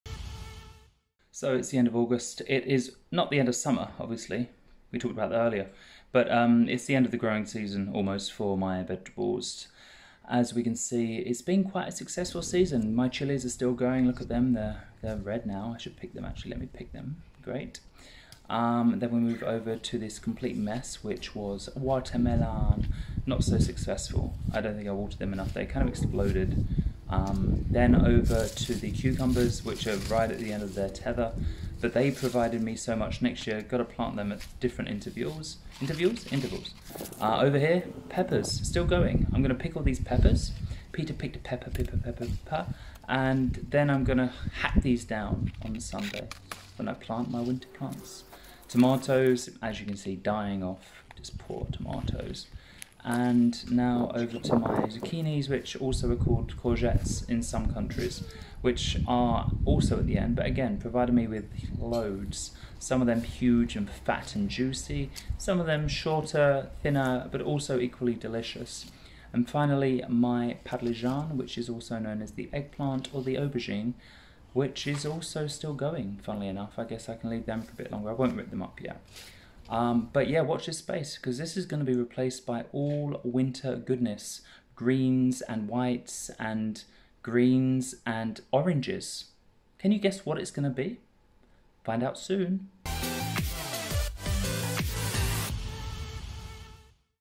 How does your garden grow?
End of season gardening means harvesting and hacking down. Before planting new for new growth. Winter plants are next. I told you the colours, you tell me the veg.

Got an anonymous question?
I'll answer it one day.